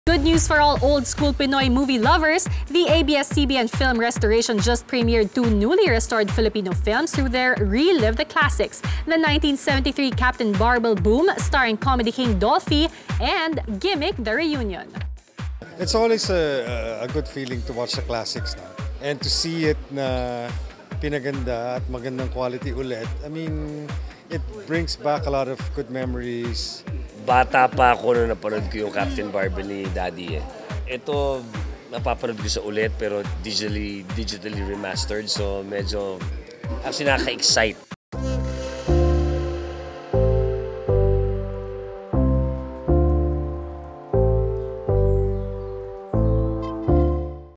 CINEMANEWS: Captain Barbell Boom and Gimik the Reunion now digitally remastered
ABS-CBN Film Restoration has successfully digitally remastered two new movies. Captain Barbell Boom starring comedy king Dolphy and 90s icon Gimik crew in Gimik the Reunion.